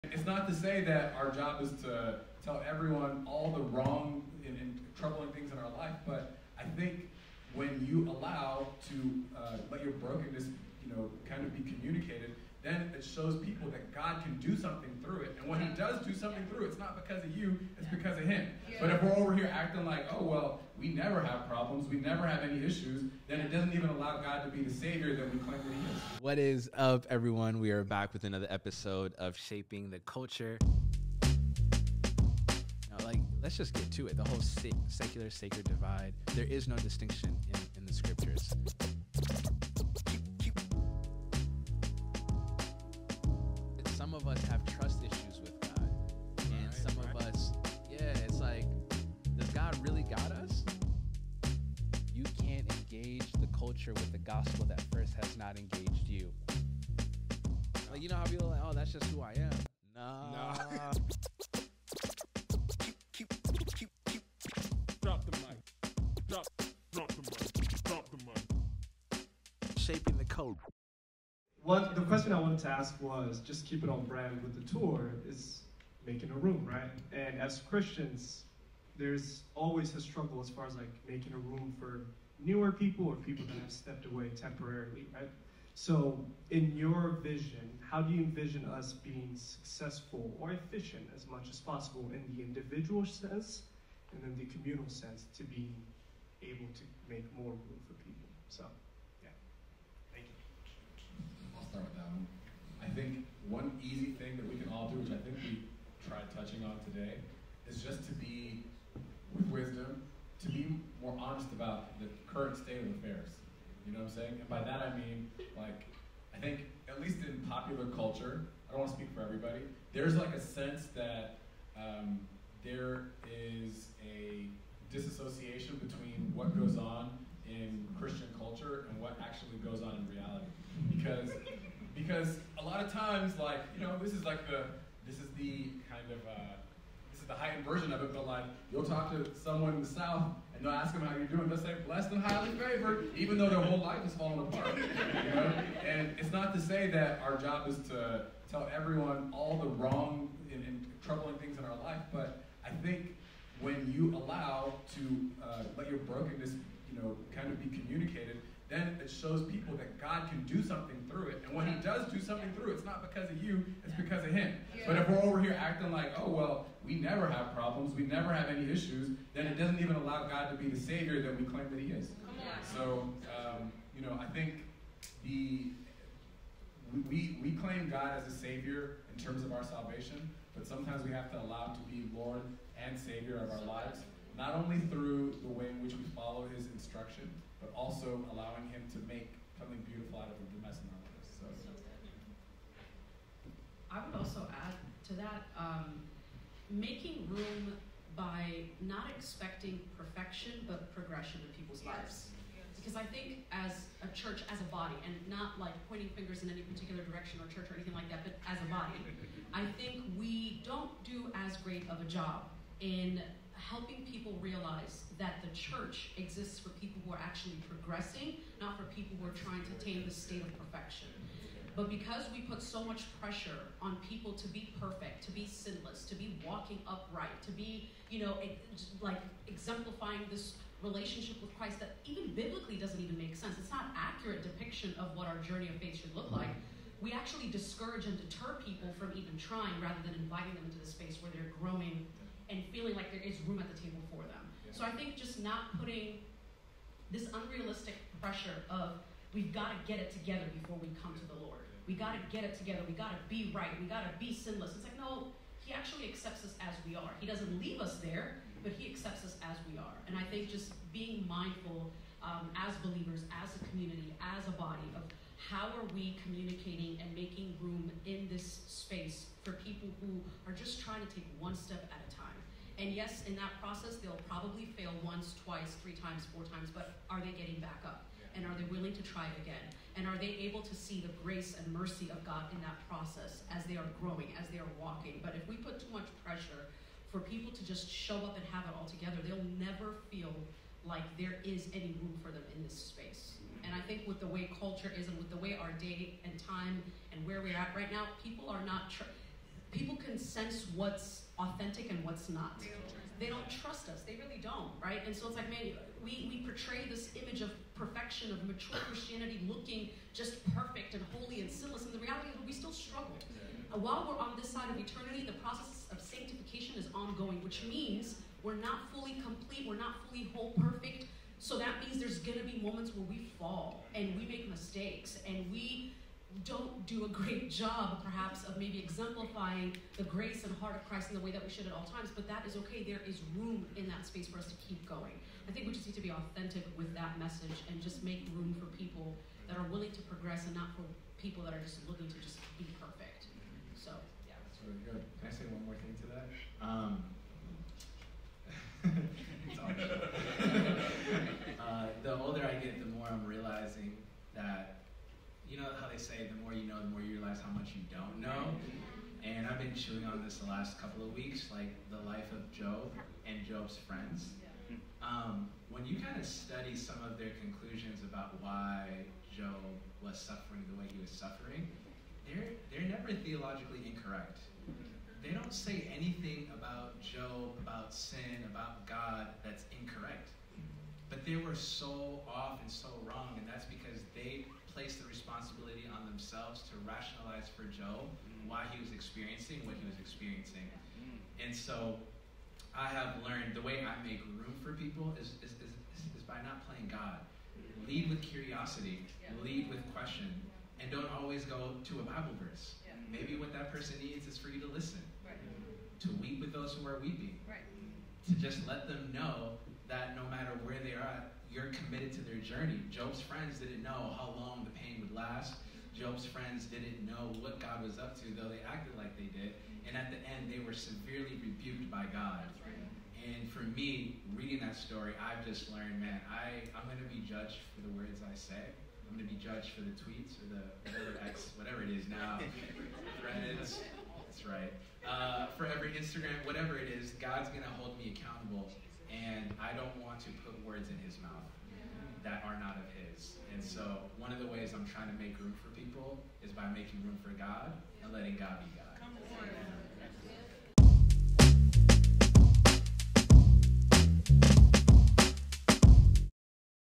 Showing Gods Love Means Being Quiet
Earlier this year we got to go on tour with some friends! Like always one of the best parts of the night is Q & A. What's a question you would have asked?

“We dive deep into the inner workings of all things culture & Christian faith and the confluence at which they intersect”